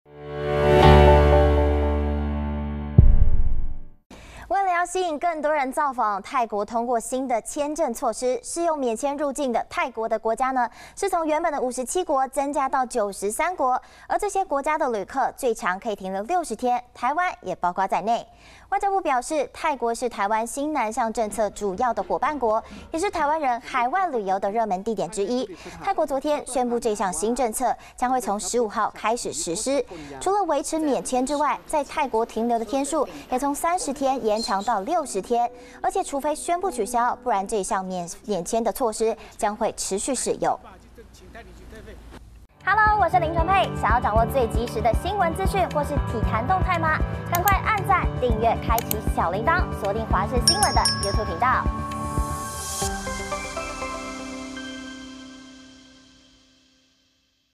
台旅客泰國免簽30天延長為60天 7/15起實施｜華視新聞 20240713
為了吸引更多人造訪，泰國通過新的簽證措施，適用「免簽入境」泰國的國家，從原本的57國，增加到93國，這些國家的旅客最長可停留60天，台灣也包括在內！